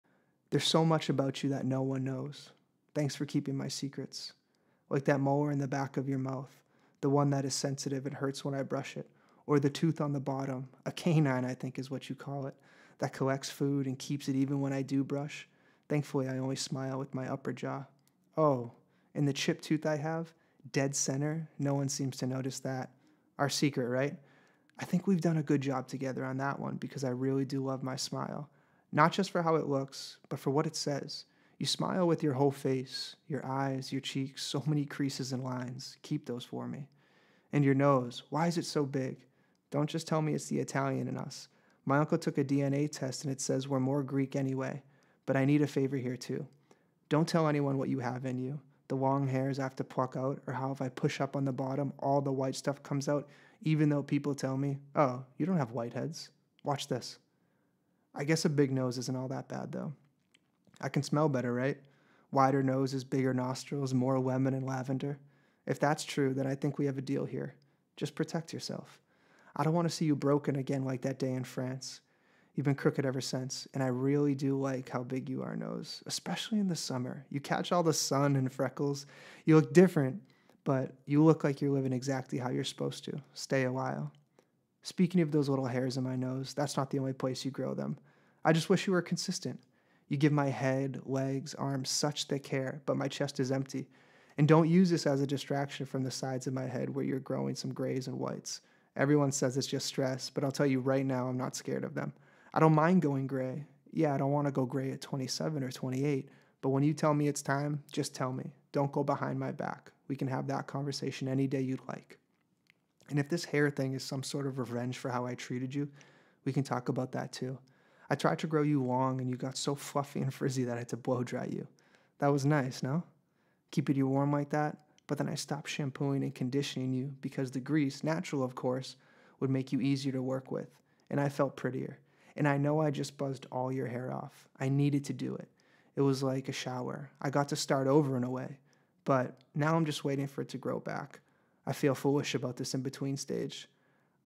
A Love Letter To My Body | Episode 4
I’d love to share more intimate ideas and moments with you, or simply open a friendship. If you’re in, click through here and buckle up

There is so much about you that no one knows. Thanks for keeping my secrets. Like that molar in the back of your mouth. The one that is sensitive, and hurts when I brush it. Or the tooth on the bottom, a canine I think is what you call it, that collects food and keeps it even when I do brush. Thankfully I only smile with my upper row.

 Oh, and the chipped tooth I have. Dead center, no one seems to notice that. Our secret right? I think we’ve done a good job together on that one because I really do love my smile. Not just for how it looks, but for what it says. You smile with your whole face. Your eyes, your cheeks, so many creases and lines. Keep those for me.

And your nose. Why is it so big? Don’t just tell me it’s the Italian in us, My uncle took a DNA test and it says we’re more greek anyway. But I need a favor here too. Don’t tell anyone what you have in you. The long hairs I have to pluck out, or how if i push up on the bottom all the white stuff comes out even though people tell me “Oh, you don’t have whiteheads” - Watch this. I guess a big nose isn’t all that bad though. I can smell better right? Wider noses, bigger nostrils, more lemon and lavender. If that's true, then I think we have a deal here. Just protect yourself, I don’t want to see you broken again like that day in France. You’ve been crooked ever since. I think I really do like how big you are nose. Especially in the summer. You catch all the sun and freckles. You look different but you look like you’re living exactly how you're supposed to. Stay a while.

Speaking of those little hairs in my nose. That’s not the only place you grow them, I just wish you were consistent.  You give my head, legs and arms such thick hair, but my chest is empty.  And don’t use this as a distraction from the sides of my head where you’re growing some greys and whites. Everyone says it’s just stress but I’ll tell you right now I’m not scared of them. I don’t mind going grey. Yeah, I don’t want to go grey at 27 or 28 but when you tell me it's time, just tell me. Don’t go behind my back. We can have that convo any day you’d like.

And if this hair thing is some sort of revenge for how I treated you we can talk about that too. I tried to grow you long and you got so fluffy and frizzy so that I had to blowdry you, that was nice no? Keeping you warm like that? But then I stopped shampooing and conditioning you because the grease, natural of course, would make you easier to work with and I felt prettier. And I know I just buzzed all your hair off. I needed to do it. It was like a shower, I got to start over in a way, but now I’m just waiting for it to grow back. I feel foolish about this in-between stage.